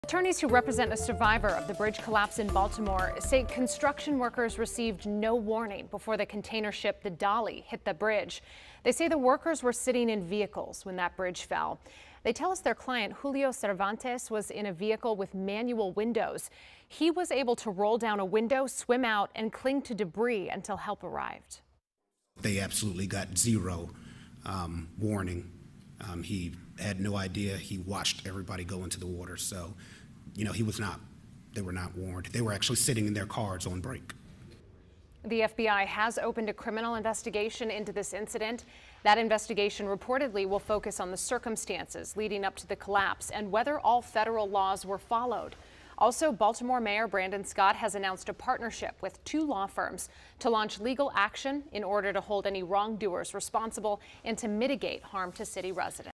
Construction workers received no warning prior to Baltimore bridge collapse, attorneys say
ATTORNEYS WHO REPRESENT A SURVIVOR OF THE BRIDGE COLLAPSE IN BALTIMORE SAY CONSTRUCTION WORKERS RECEIVED NO WARNING BEFORE THE CONTAINER SHIP DALI HIT THE BRIDGE.